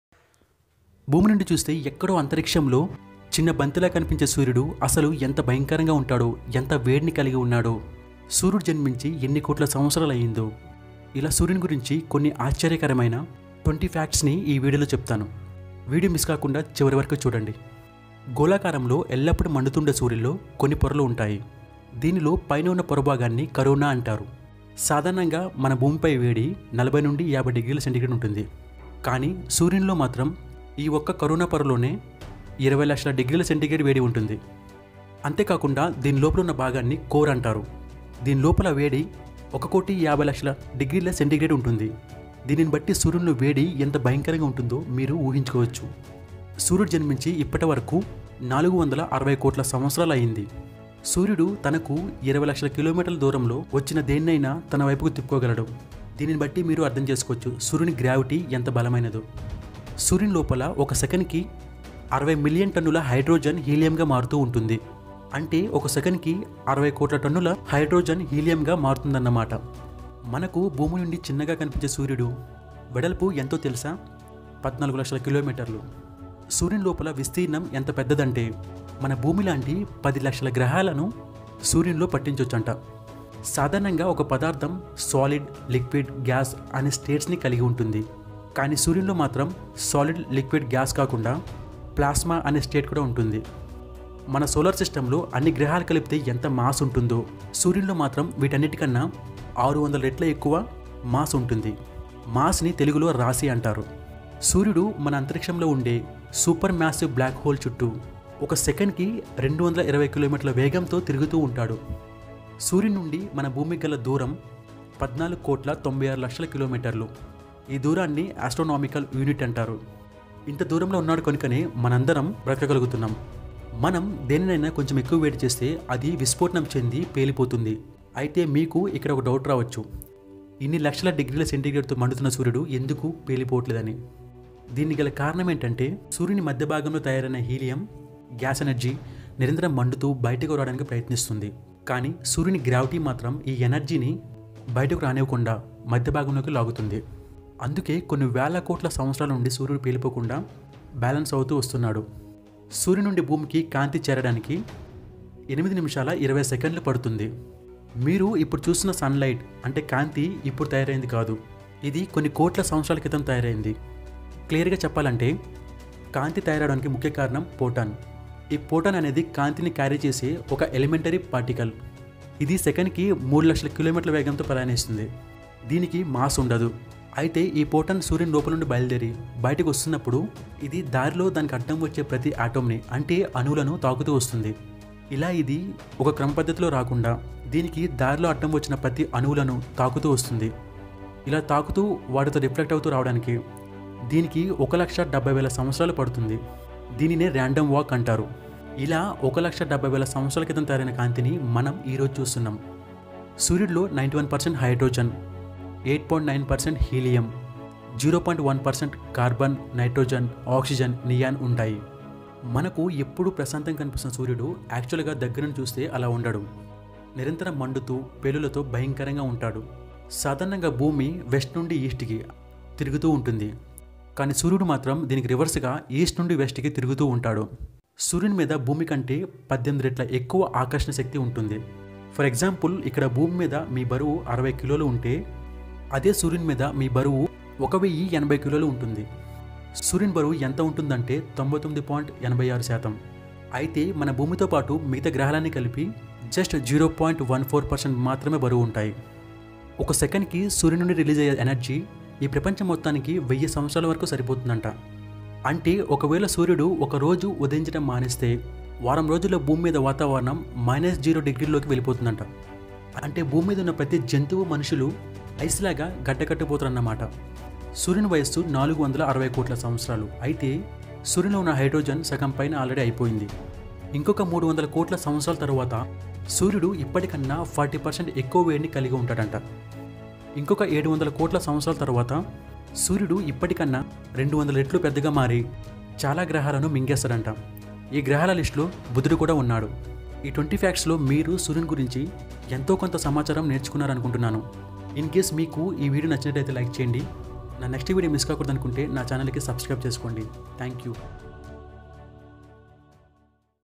20 amazing facts about sun ☀!! BRT (B. RAVITEJA) Telugu channel !!
20 amazing facts about sun ☀!! BRT             (B. RAVITEJA) Telugu channel !!

About sun, interesting facts, amazing facts,
Best facts, BRT Telugu channel, RAVITEJA Channel. here i made a video about sun
It will give you more knowledge